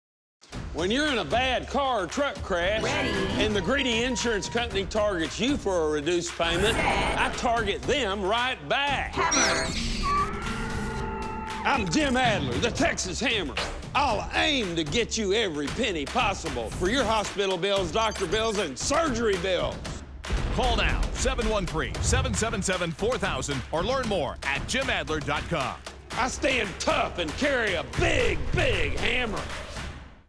Jim Adler - Bullseye on Insurance Compensation 30s (Houston 1)
When you’re in a bad car
or truck crash and the greedy insurance company
targets you for a reduced payment,
I target them right back!
I’m Jim Adler The Texas Hammer!
I’ll aim to get you every penny possible for your hospital bills, doctor bills and surgery bills.
I stand tough and carry a big Big Hammer!